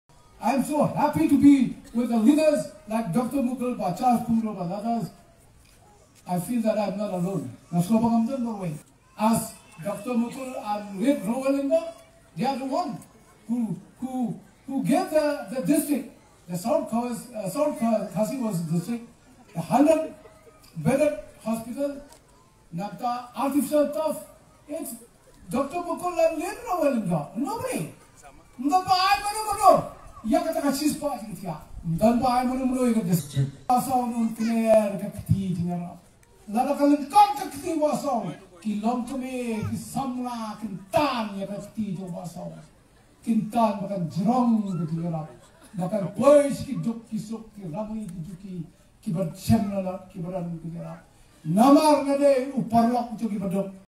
Mr. Sounder S. Cajee, expresses the importance of visionary leadership for Meghalaya
"I’m so happy to be with leaders like Dr. Mukul, Bah Charles and others. I feel like I don’t stand alone!"

— Mr. Sounder S. Cajee, expressing the importance of progressive, and visionary leadership for Meghalaya's brighter future.

Together, we will prosper!